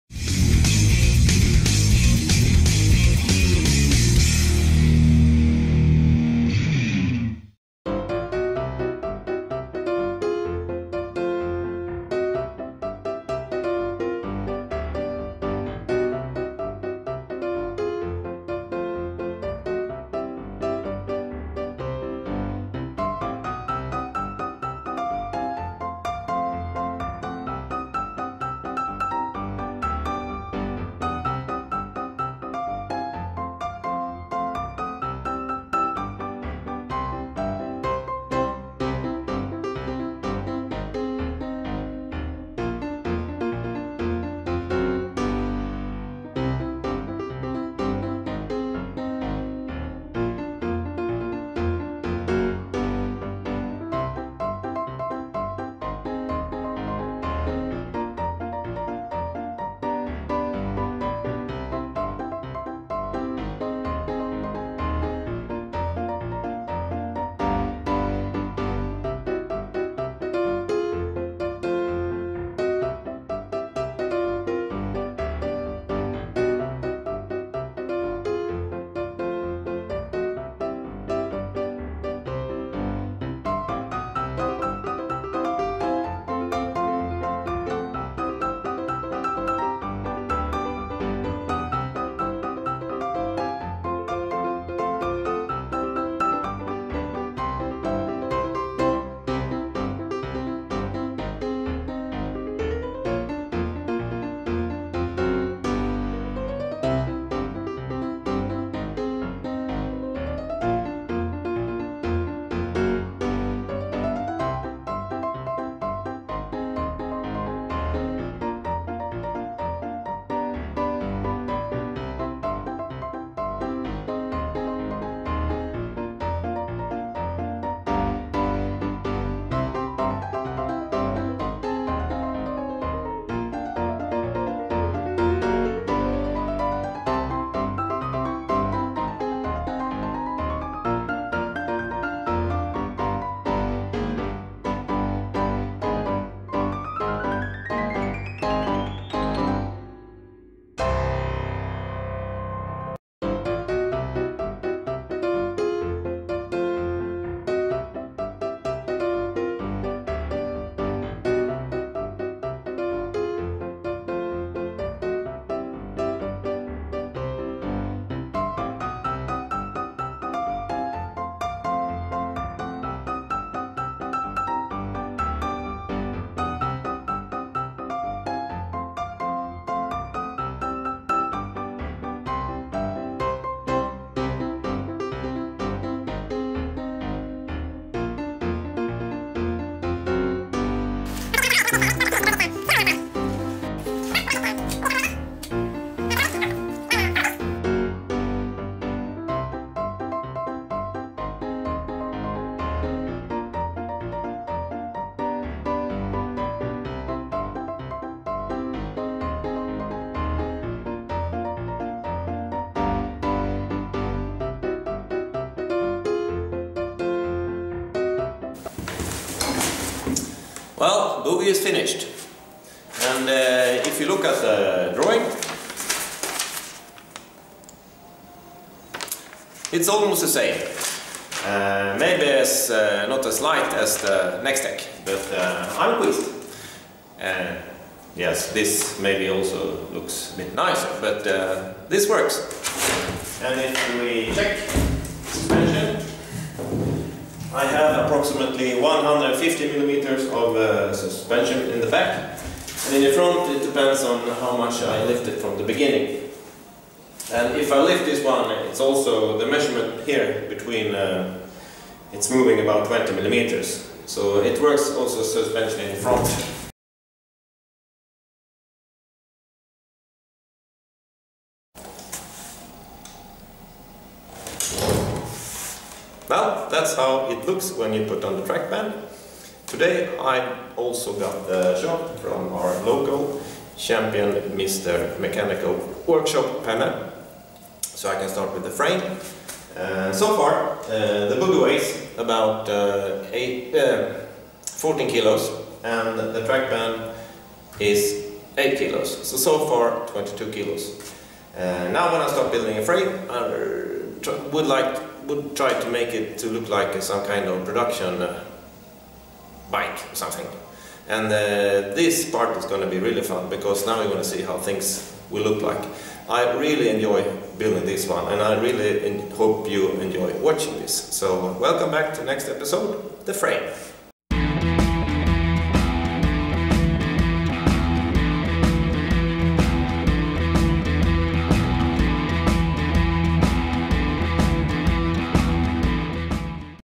Electric Snowcross 2. Boggie Suspension
Electric Snowcross rebuild.  ME1115 motor Kelly Controller and DIY 96V 400A batterypack.  Monoboggie, Alpha One, Nextech design.
Polaris Snowmobile parts and Yamaha YZ chassie.